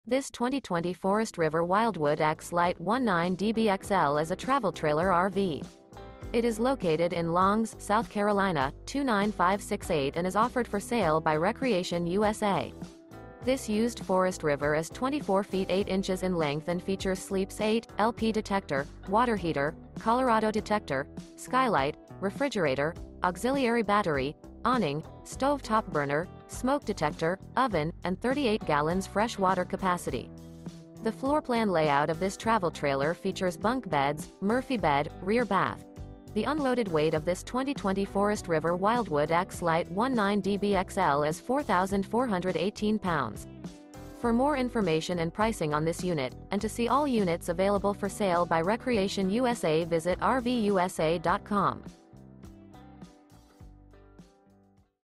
Breathtaking 2020 Forest River Wildwood X-Lite Travel Trailer RV For Sale in Longs, SC | RVUSA.com
This 2020 Forest River Wildwood X-Lite 19DBXL is a Travel Trailer RV. It is located in Longs, SC 29568 and is offered for sale by Recreation USA. This Used Forest River Is 24' 8" in length and features sleeps 8, Skylight, Auxiliary Battery, Oven, Leveling Jacks, Awning, Stove Top Burner, LP Detector, CO Detector, Microwave, Water Heater, and 38 gal fresh water capacity. The floorplan layout of this Travel Trailer features Bunk Beds, Murphy Bed, Rear Bath.The unloaded weight of this 2020 Forest River Wildwood X-Lite 19DBXL is 4418 lbs.